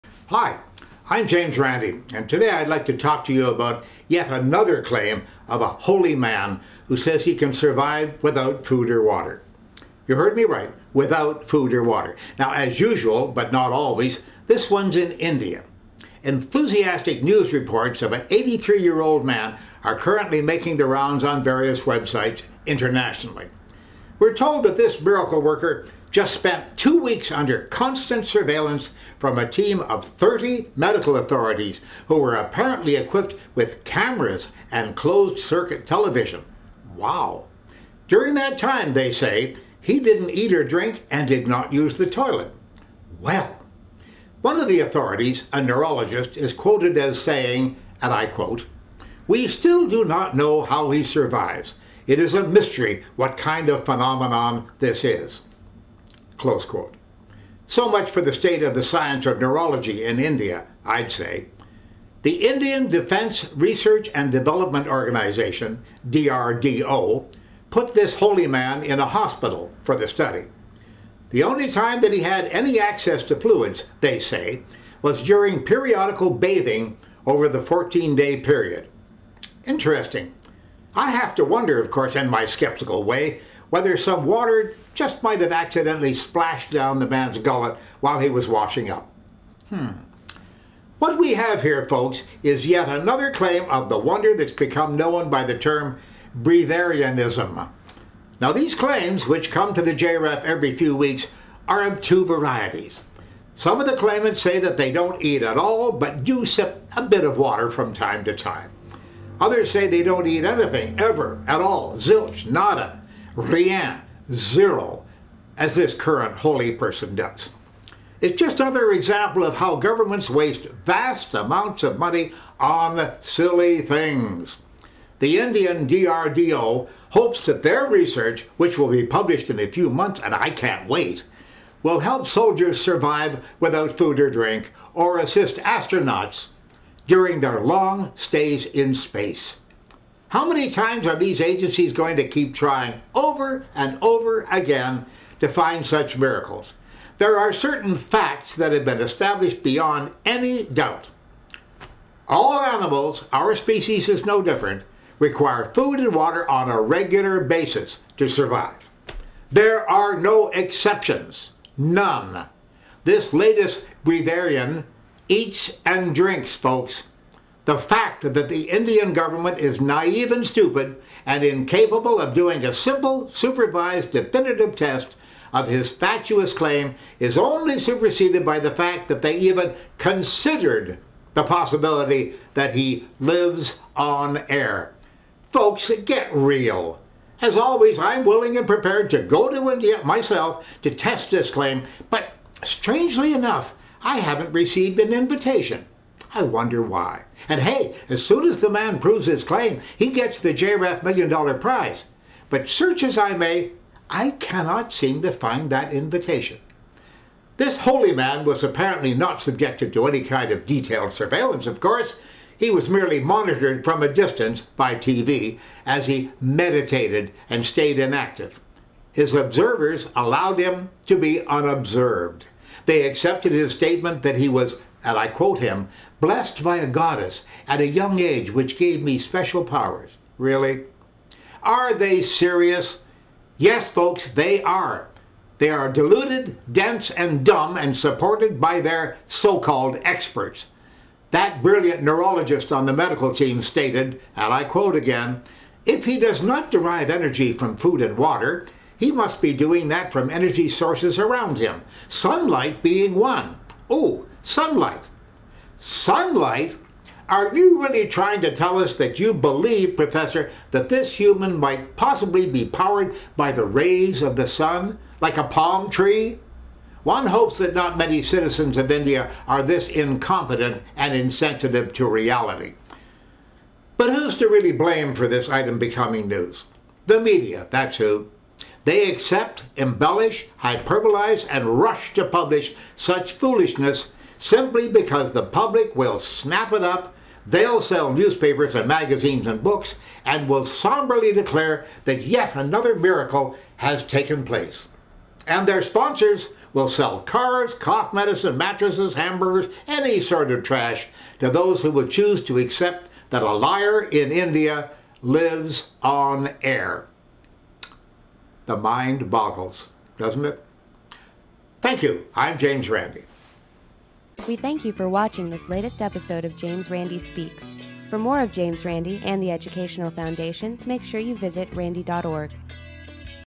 James Randi Speaks: Powered by Sunlight
Recorded 5/11/2010 at the James Randi Educational Foundation in Fort Lauderdale, FL.

Can a person survive on just air? Or maybe as a professor in a recent study says "If he is not derive energy from food and water, he must be doing that from energy sources around him, sunlight being one

There have been many claimants over the years for the Million Dollar Challenge who say that they have survived for years without ingesting any food or water. Unfortunately when they are really put to the test they fail.